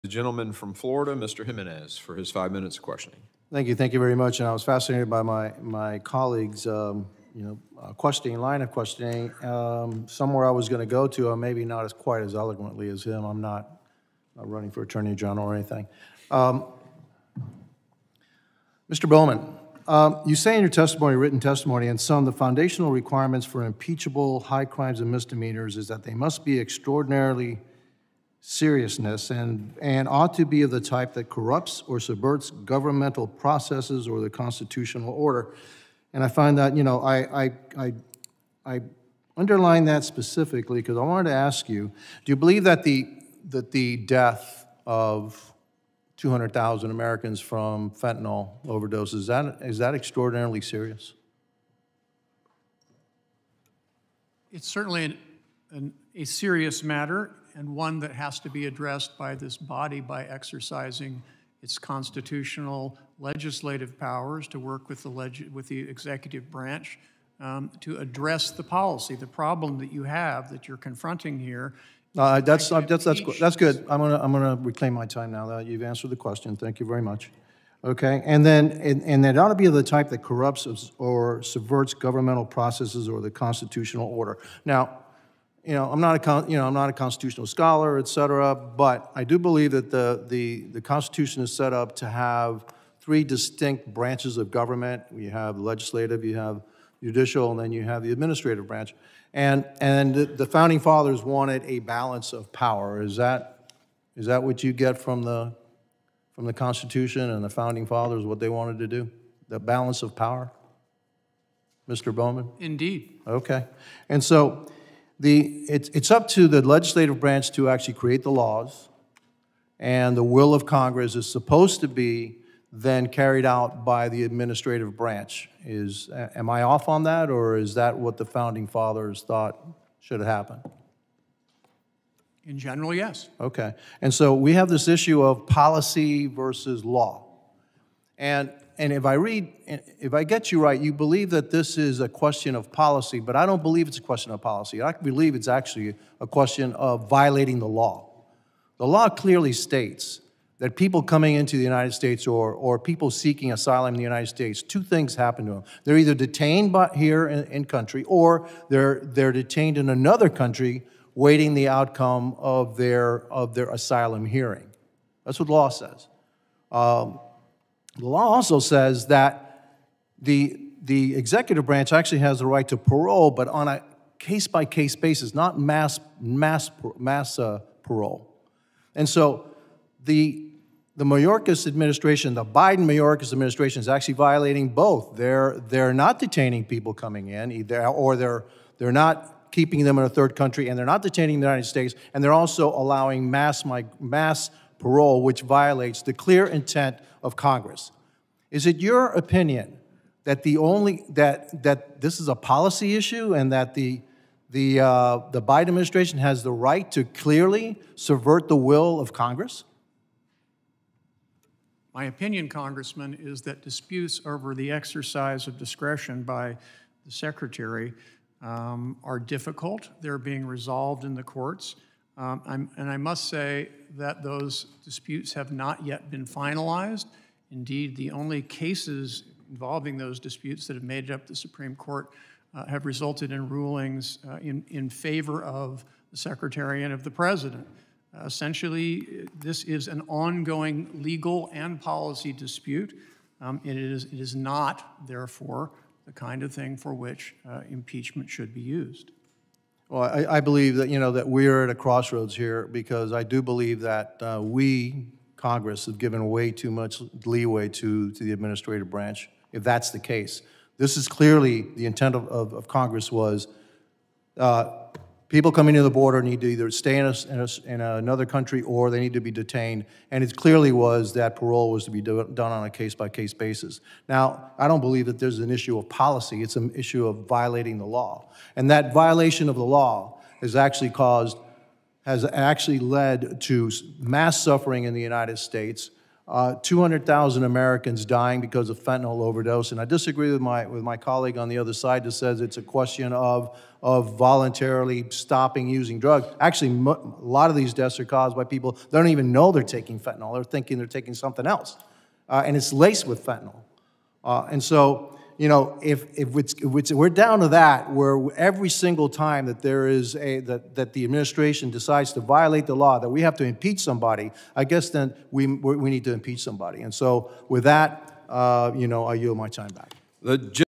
‘We Need To Impeach Somebody’: Carlos Gimenez Accuses Biden & Mayorkas Of Violating Laws At Border
During a House Homeland Security Committee hearing earlier this month, Rep. Carlos Gimenez (R-FL) questioned witnesses about the need to impeach Sec. Alejandro Mayorkas.